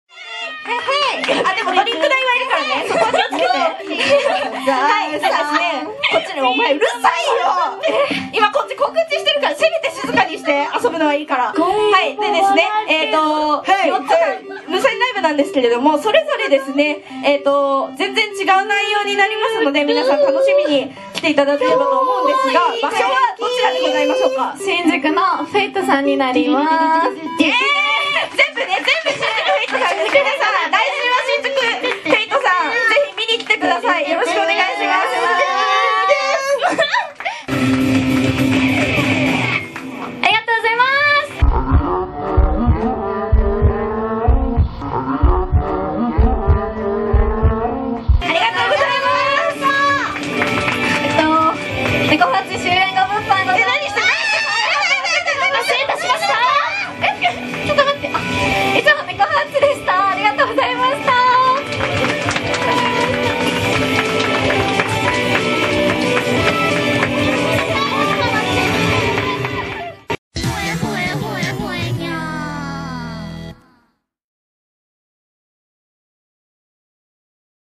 ［羞恥！］ライブ中にお尻が見えてしまったハプニング映像。【ねこふわっち。】 ライブアイドル
★【ねこふわっち。】ライブアイドル★

チェンネル登録よろしくにゃん！

Take​​ off
Neko​​​​-Huwacchi
Live​​​​ idol